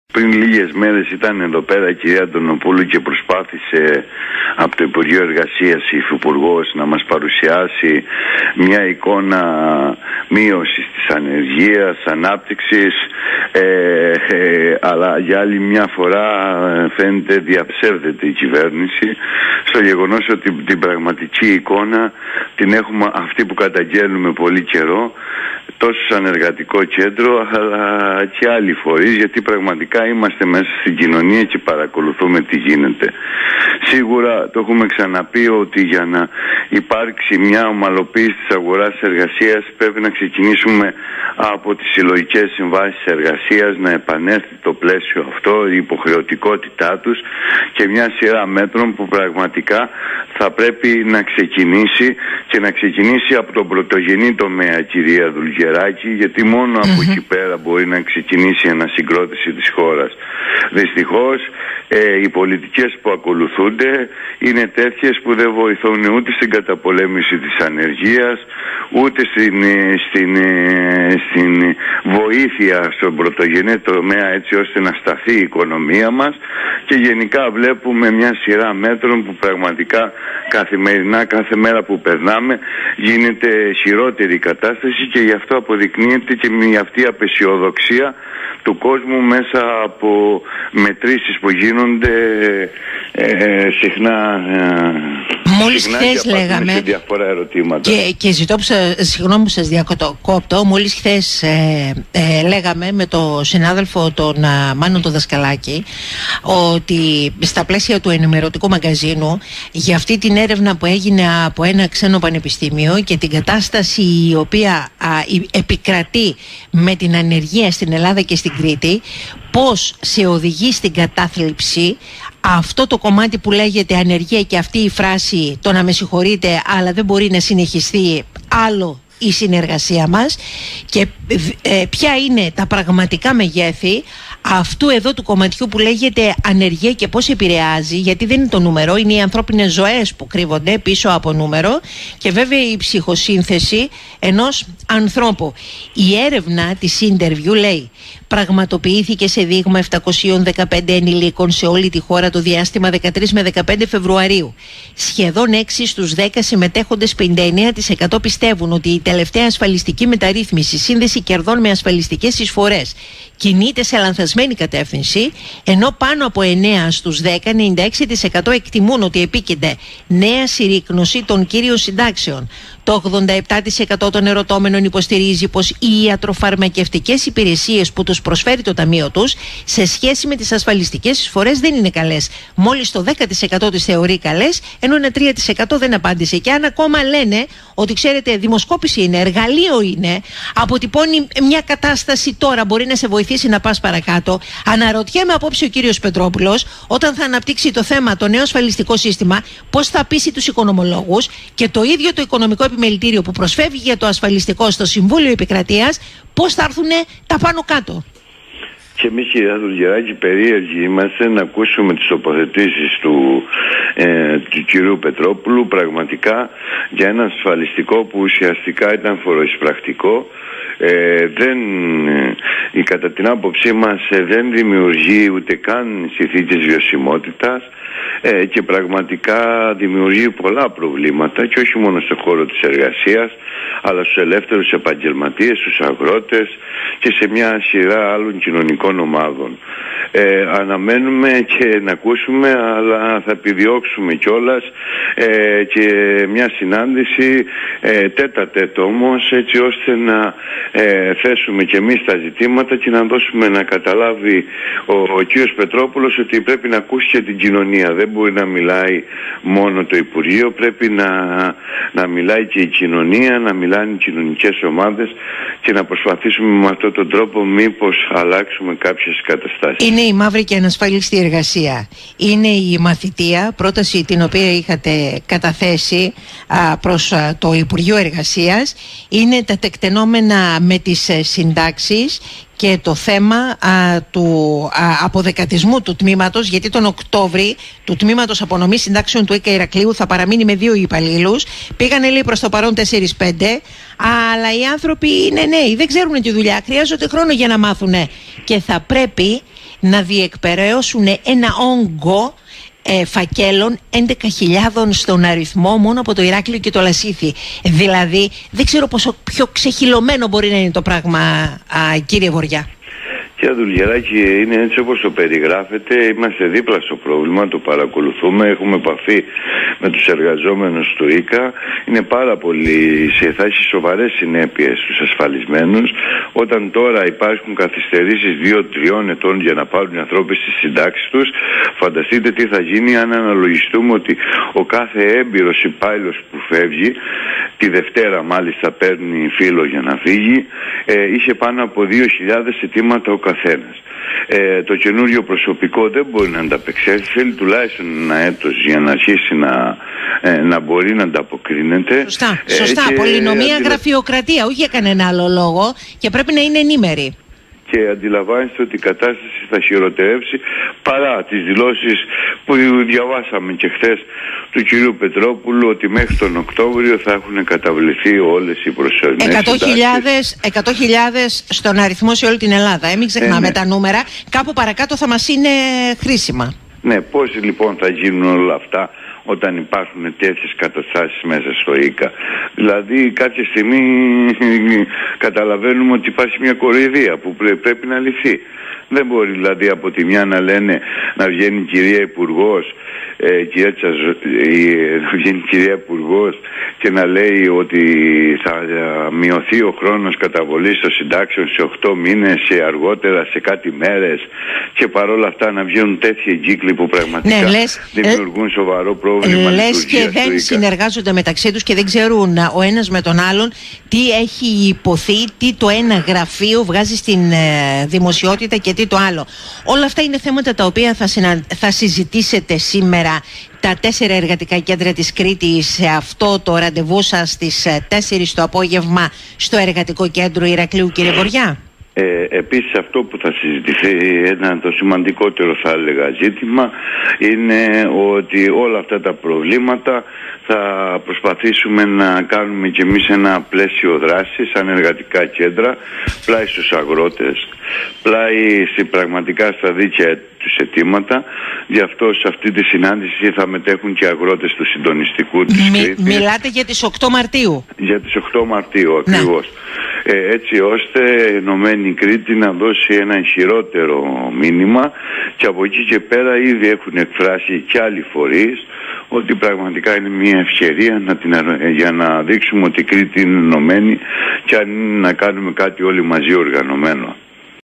Συνάντηση των Εργατικών κέντρων Κρήτης στο Ηράκλειο
Στις 16:00 το απόγευμα στο Εργατικό Κέντρο Ηρακλείου, θα πραγματοποιηθεί η συνάντηση των τεσσάρων Εργατικών Κέντρων Κρήτης με τρέχοντα εργασιακά ζητήματα (μαθητεία στα Ξενοδοχεία, κατάθεση μισθοδοσίας μέσω τραπέζης, υποστελέχωση Σ.ΕΠ.Ε, ΙΚΑ κ.α ), όσο και για το θέμα κινητοποίησης των Αγροτών – κοινές δράσεις.
 ‘’Για την ομαλοποίηση της αγοράς εργασίας απαιτείται η επαναφορά των συλλογικών συμβάσεων εργασίας, η αντιμετώπιση της μαύρης και ελαστικής εργασίας και όχι τα πολιτικά παιγνίδια του κάθε γραφείου’’ είπε στο Ράδιο 9,84 ο πρόεδρος του ΕΚΗ Στέλιος Βοργιάς.
Ο ίδιος επίσης ανέφερε ότι το ΕΚΗ ζητά συνάντηση με τον υφυπουργό Τάσο Πετρόπουλο που σήμερα κι αύριο θα βρίσκεται στο Ηράκλειο καθώς απαιτούνται άμεσα λύσεις στα θέματα της περίφημης μαθητείας, της μαύρης και ελαστικής εργασίας, της αποδυνάμωσης του τμήματος απονομής συντάξεων στο ΙΚΑ Ηρακλείου με 11.000 φακέλους απονομής σύνταξης να παραμένουν σε εκκρεμότητα.